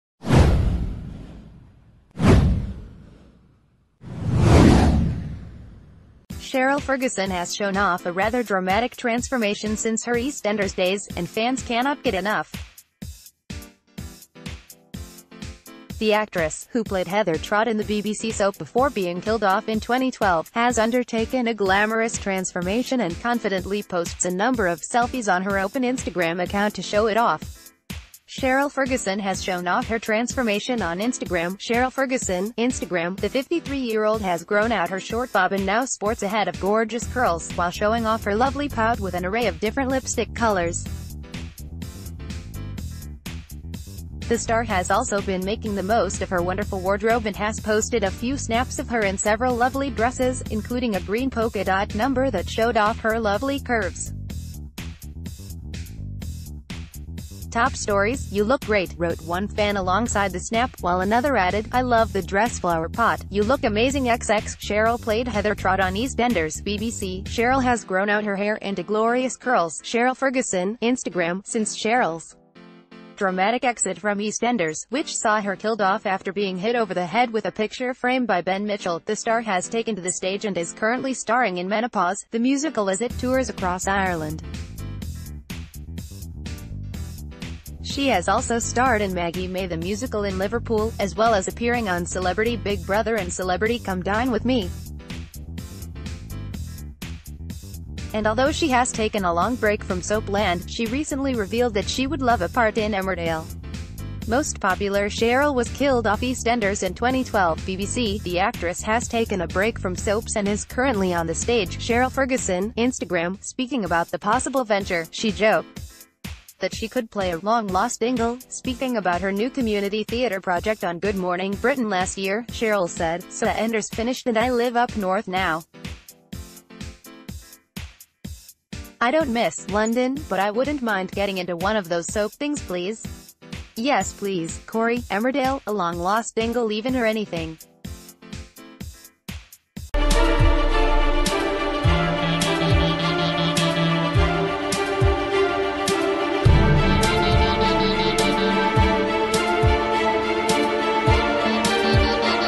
EastEnders star Cheryl Fergison shows off dramatic transformation
Cheryl Fergison has shown off a rather dramatic transformation since her EastEnders days, and fans cannot get enough. The actress, who played Heather Trott in the BBC soap before being killed off in 2012, has undertaken a glamorous transformation and confidently posts a number of selfies on her open Instagram account to show it off. Cheryl Fergison has shown off her transformation on Instagram [Cheryl Fergison/Instagram] The 53 year old has grown out her short bob and now sports a head of gorgeo...